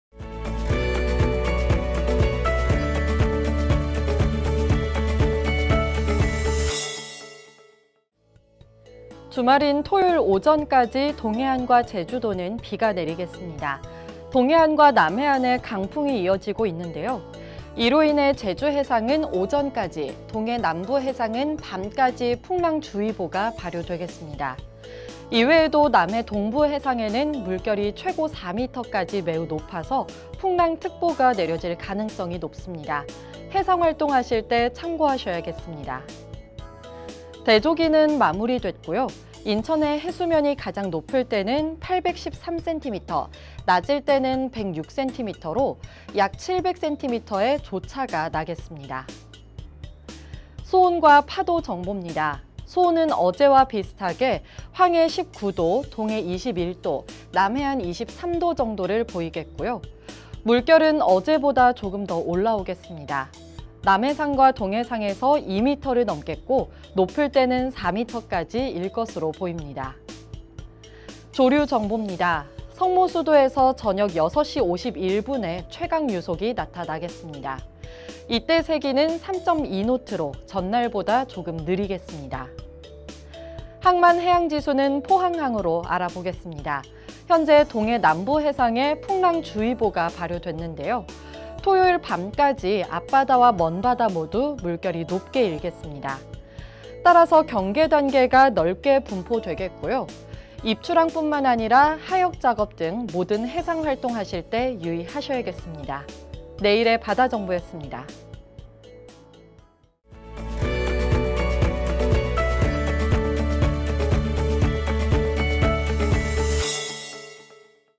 [내일의 바다정보] 2019년 10월 19일
▶ 기사 원문 :
주말인 토요일 오전까지 동해안과
제주도는 비가 내리겠습니다.
동해안과 남해안에 강풍이 이어지고 있는데요.
이로 인해 제주해상은 오전까지, 동해남부해상은
밤까지 풍랑 주의보가 발효되겠습니다. 이외에도
남해동부 해상에는 물결이 최고 4미터까지 매우 높아서
풍랑특보가 내려질 가능성이 높습니다.
해상활동 하실 때 참고하셔야겠습니다.

대조기는 마무리됐고요.
인천의 해수면이 가장 높을 때는 813cm,
낮을 때에는 106cm로 약 700cm의 조차가 나겠습니다.

수온과 파도정보입니다.
수온은 어제와 비슷하게 황해 19도, 동해 21도,
남해안 23도 정도를 보이겠고요.
물결은 어제보다 조금 더 올라오겠습니다.
동해상과 남해상에서 2m를 넘겠고,
높을 때는 4m까지 높게 일 것으로 보입니다.

조류정보입니다.
석모수도에서 저녁 6시 51분에
최강유속이 나타나겠습니다.
이때 세기는 3.2노트로 전날보다 조금 느리겠습니다.

항만해양지수는 포항항으로 알아보겠습니다.
현재 동해남부해상에 풍랑 주의보가 발효됐는데요.
토요일 밤까지 앞바다와 먼바다 모두 물결이 높게 일겠습니다.
따라서 경계단계가 넓게 분포되겠고요.
입출항뿐만 아니라 하역작업 등 모든
해상활동하실 때 유의하셔야겠습니다.

내일의 바다정보였습니다.